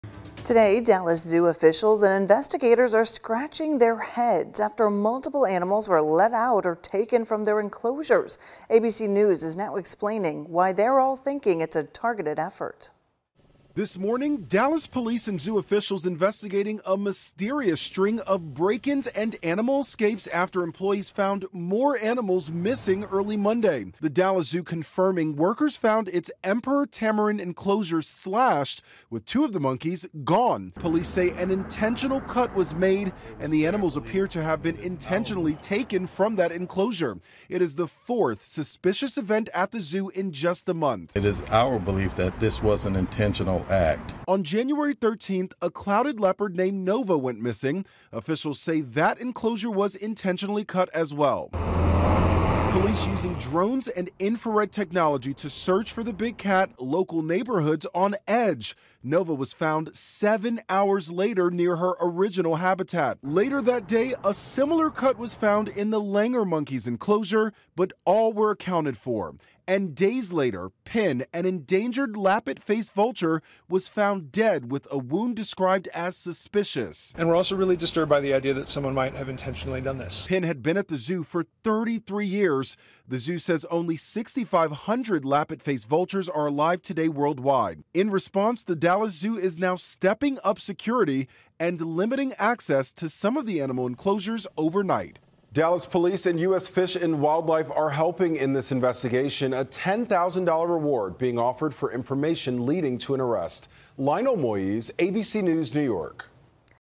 Officials investigating string of break-ins, animal escapes at Dallas Zoo
The latest instance was an "intentional cut" in the Emperor Tamarin enclosure and two of the monkeys missing. This is the fourth suspicious event in a month.